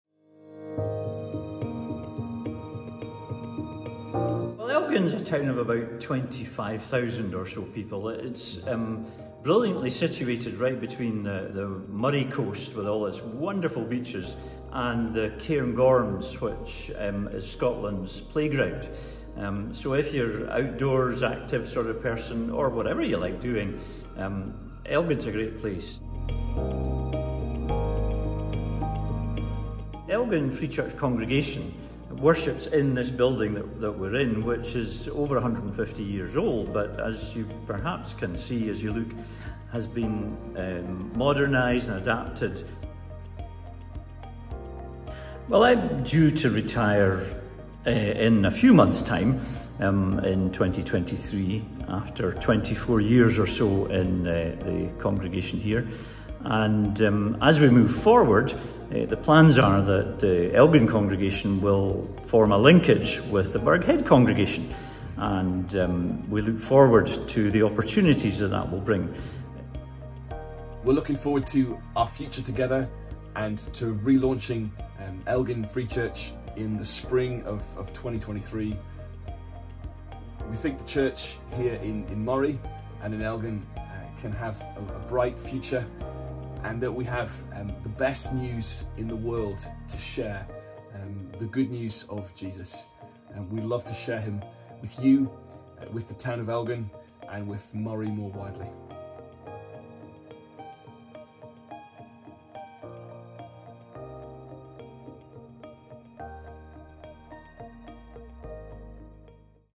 Elgin, Forres and Burghead Free Church - short introduction
A short introduction to the linkage planned between Elgin and Burghead Free Churches in Spring 2023 - as we relaunch the Elgin congregation.